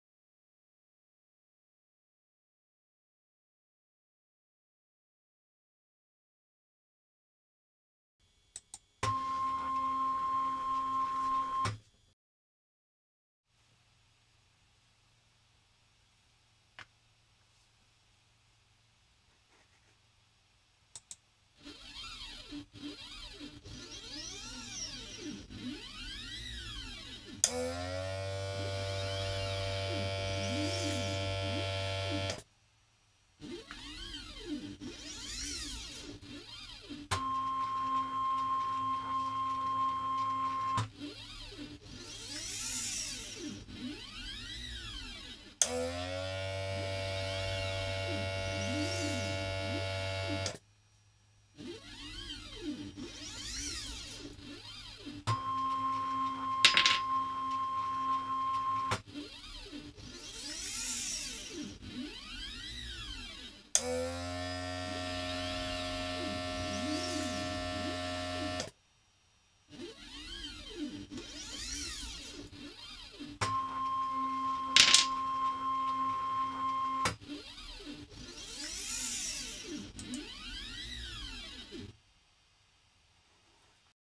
opdracht 6 Start and Stop Conveyor
In deze video zie je de uitwerking van opdracht 6 van de cursus Robotica 2